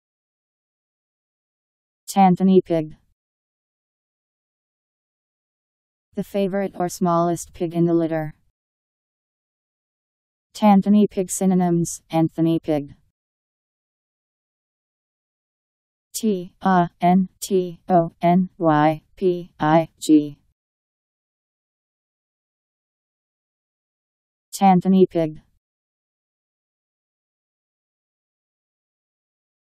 Tantony pig Meaning
Video shows what tantony pig means. The favourite or smallest pig in the litter.. tantony pig synonyms: Anthony pig.  Tantony pig Meaning. How to pronounce, definition audio dictionary. How to say tantony pig. Powered by MaryTTS, Wiktionary